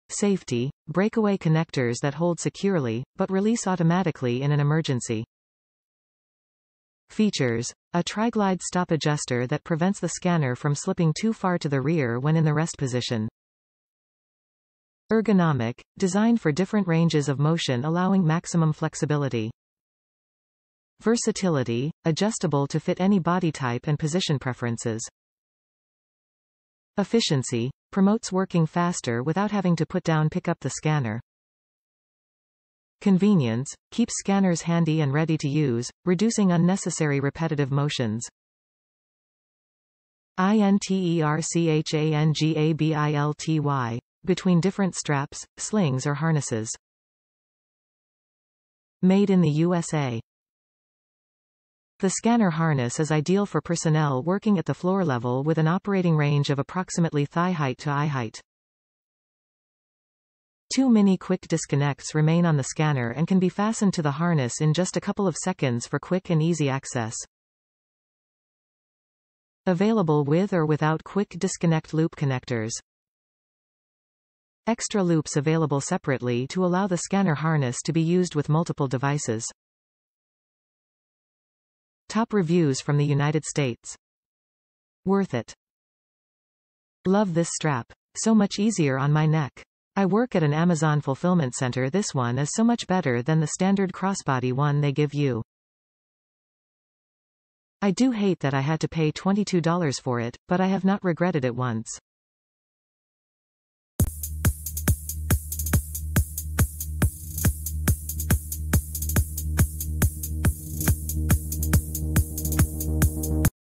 OP/TECH USA Warehouse Scanner Harness with Breakaway Buckles (Large) 99013913
SAFETY – Breakaway connectors that hold securely, but release automatically in an emergency
FEATURES - a triglide stop adjuster that prevents the scanner from slipping too far to the rear when in the rest position
ERGONOMIC – designed for different ranges of motion allowing maximum flexibility
VERSATILITY – adjustable to fit any body type and position preferences
EFFICIENCY – promotes working faster without having to put down & pick up the scanner
CONVENIENCE – keeps scanners handy and ready to use, reducing unnecessary repetitive motions
INTERCHANGABILTY - between different straps, slings or harnesses
The SCANNER HARNESS is ideal for personnel working at the floor level with an operating range of approximately thigh-height to eye-height. Two mini quick disconnects remain on the scanner and can be fastened to the harness in just a couple of seconds for quick and easy access. Available with or without quick disconnect loop connectors. Extra Loops available separately to allow the SCANNER HARNESS to be used with multiple devices.
Worth it.
Love this strap! So much easier on my neck! I work at an amazon fulfillment center & this one is so much better then the standard cross body one they give u. I do hate that I had to pay $22 for it, but I have not regretted it once